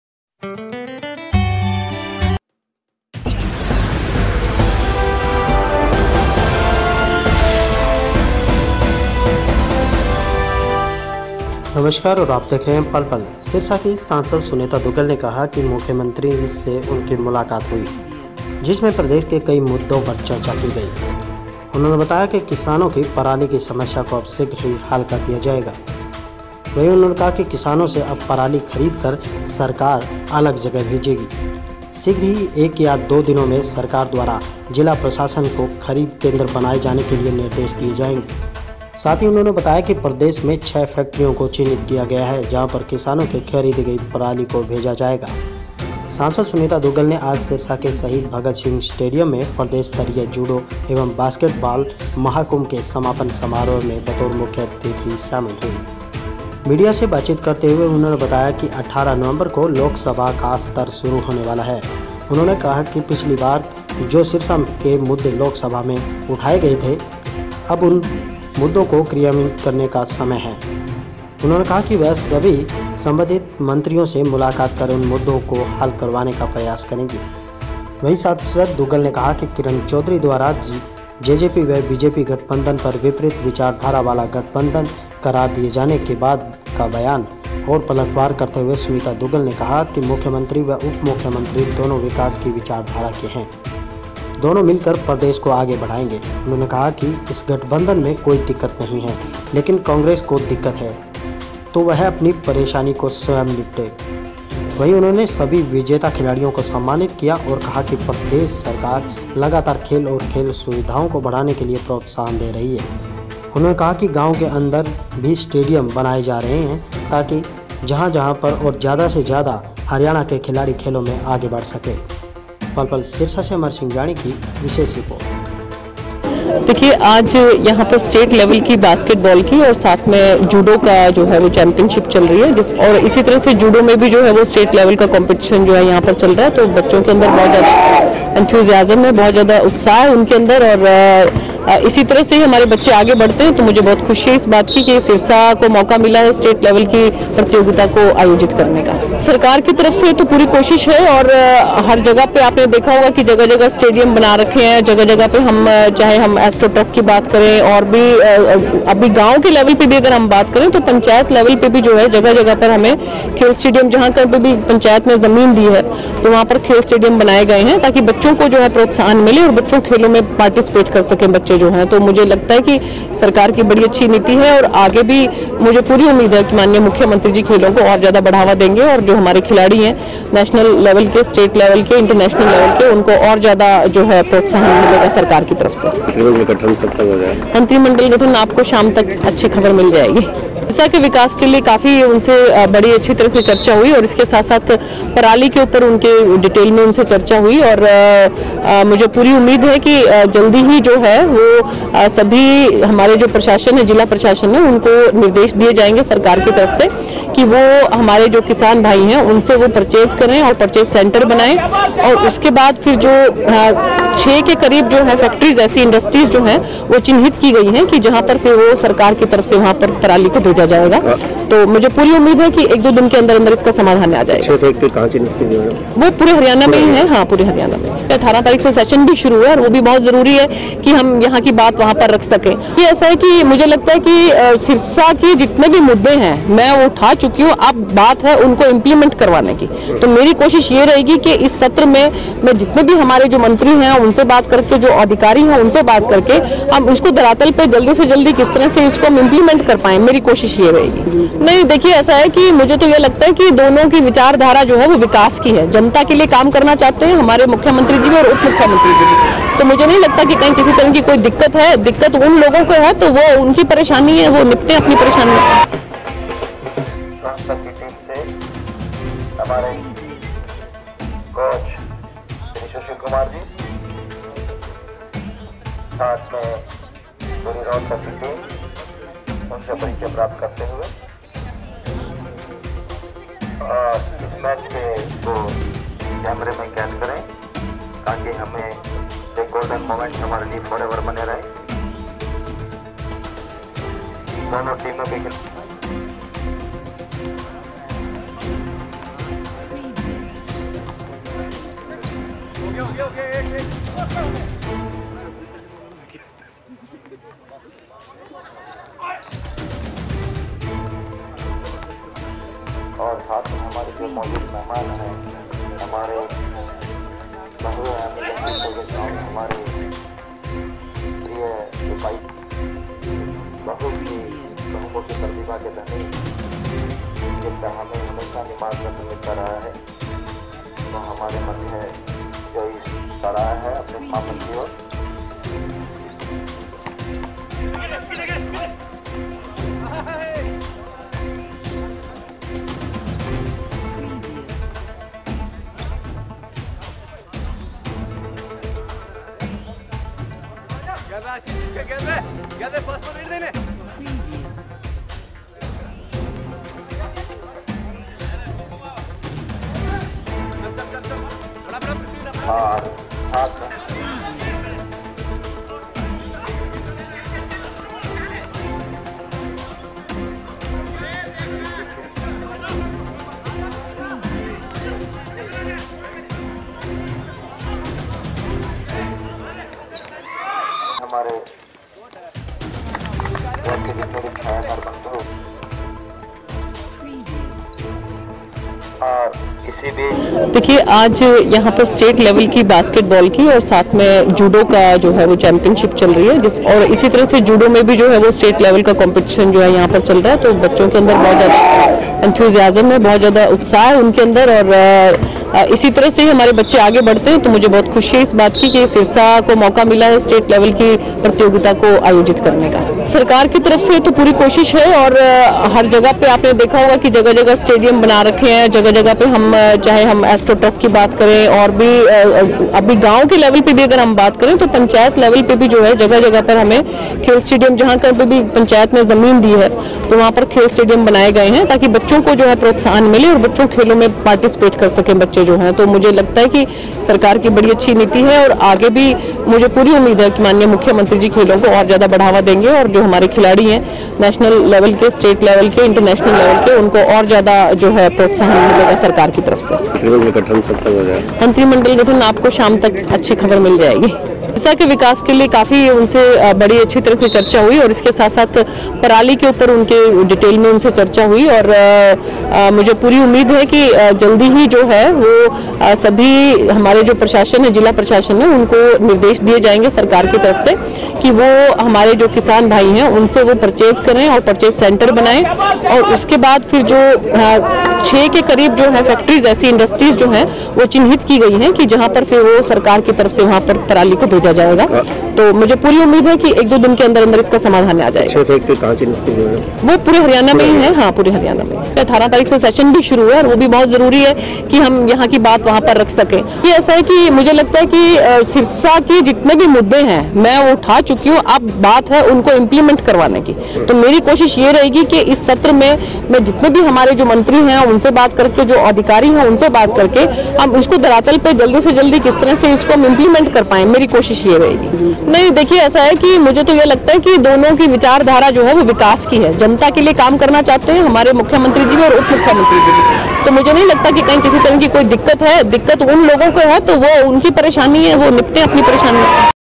दिक्कत नही, अगर कांग्रेस को कोई दिक्कत है वो निपटे अपनी परेशानी-दुग्गल
सिरसा की सांसद सुनीता दुग्गल ने  कहां है कि मुख्यमंत्री से उनकी मुलाकात हुई जिसमें प्रदेश के कई मुद्दों पर चर्चा की गई है। उन्हें बताया कि किसानों की पराली की समस्या को अब शीघ्र हल कर दिया जाएगा। उन्होंने बताया कि किसानों से अब पराली की खरीद सरकार करेगी ।उन्होंने बताया कि शीघ्र एक या दो दिनों में सरकार द्वारा जिला प्रशासन को खरीद केंद्र बनाए जाने के लिए निर्देश जारी किए जाएंगे ।साथ ही उन्होंने बताया कि प्रदेश में 6 फैक्ट्रियों को चिन्हित किया गया है जहां किसानों से खरीदी गई पराली को भेजा जाएगा।
सांसद दुग्गल आज सिरसा के शहीद भगत सिंह स्टेडियम में प्रदेश स्तरीय जुडो व बास्केटबॉल के महाकुंभ के समापन समारोह में बतौर मुख्य अतिथि शामिल हुई।